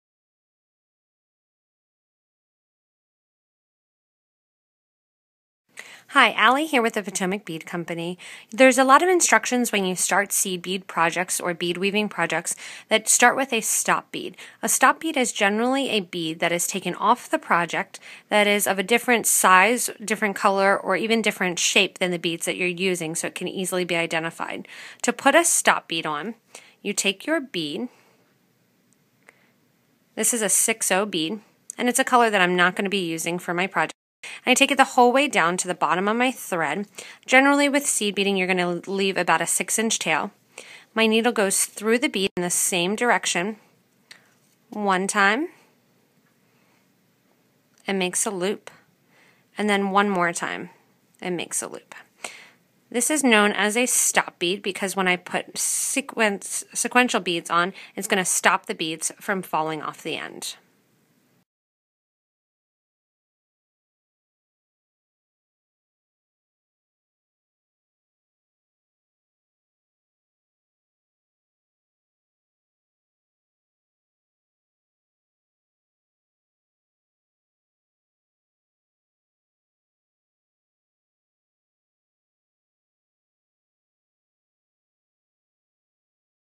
How To Make and Use a Stop Bead
This tutorial from The Potomac Bead Company demonstrates how to make or use a stop bead in beadweaving or seed bead projects.  A stop bead allows you to keep your beads from sliding off the thread, and also reminds you where you started the project.